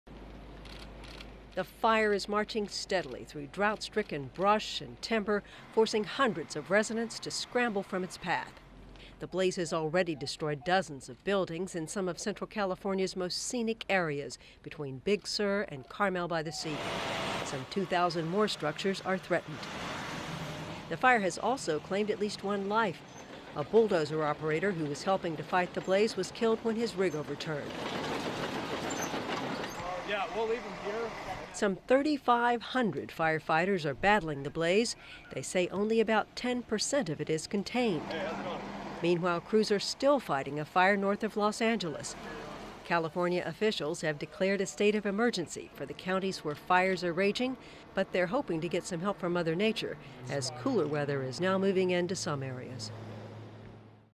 Wildfires in Big Sur destroys dozens of homes, 32K acres - Daily Mail
Incredible aerial photos have captured the devastation left behind after colossal wildfires have wiped out forests in Antelope Island in the Great Salt Lake (left main and top left) and destroyed at least 60 homes in California's Big Sur coast (top and bottom right).